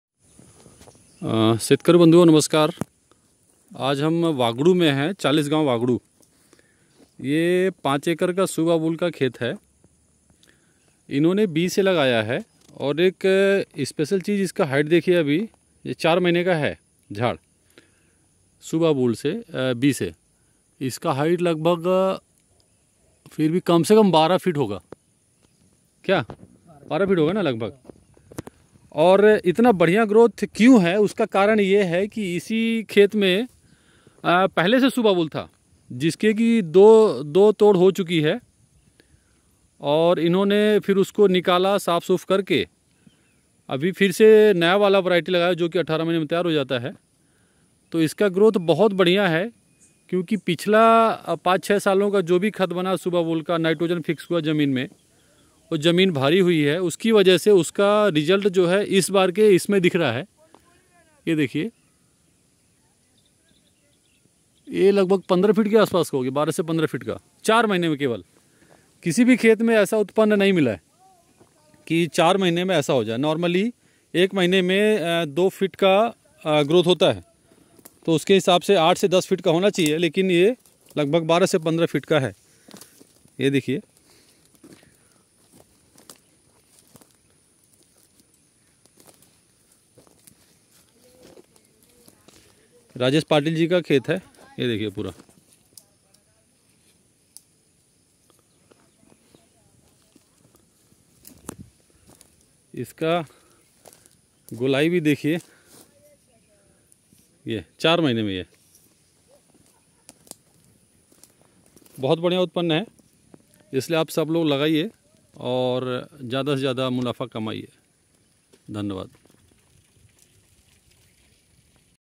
सुबबूल शेती से मिट्टी की उर्वरता बढ़ती है। ज्यादा जानकारी के लिए डिस्क्रिप्शन में देखें।
यह सुबबूल का शेत राजेश पाटिल जी का ग्राम वाघडू, तालुका, चालीसगांव जिल्हा जलगांव में है। वो नाशिक में काम पर है इसलिए उनके लिए सुबबूल की शेती सबसे बेहतर है। इन्होंने 2016 में 3 साल वाली जाति लगाई थी जिसकी दो तोड़ हो चुकी है। उसके बाद इन्होंने खेत को साफ करके नई डेढ़ साल वाली जाती लगाई है। जो कि केवल 4 महीने में  लगभग 15 फ़ीट का हो चुका है। वीडियो में देखा जा सकता है।

कोई भी राजेश जी बात कर सकते है।

जे के पेपर द्वारा सुबबूल की करार शेती की जाती है और हामी भाव के साथ साथ तोड़कर ले जाने की गारंटी और स्टाम्प पेपर एग्रीमेंट किया जाता है।